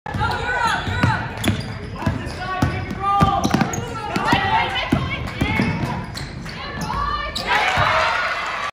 Riley Allen drives to the basket for Chatham
Riley Allen drives the lane to score for Chatham vs. Pope John